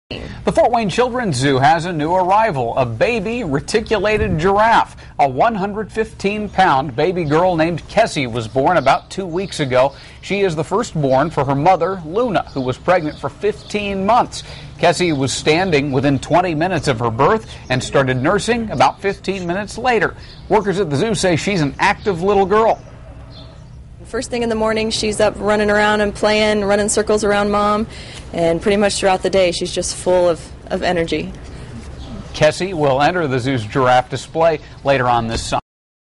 FW Children's Zoo Welcomes Baby Giraffe
The baby giraffe, a girl named Kesi, was born on Thursday, June 3.